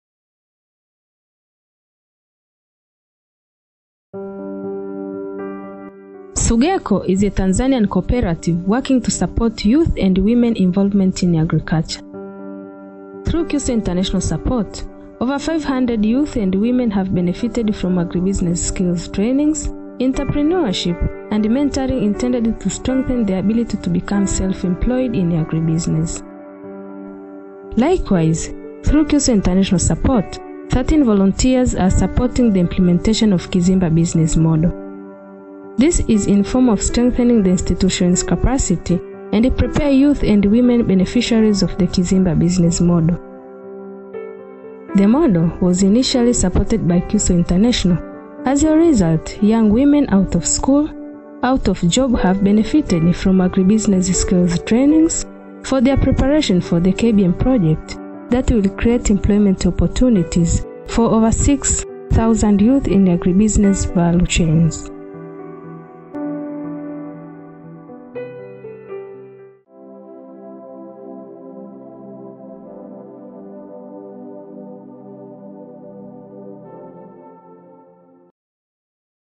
Kizimba Business Model - Supporting girls and women in Tanzania
Over 500 girls and women have benefitted through agri-skills and entrepreneurship training.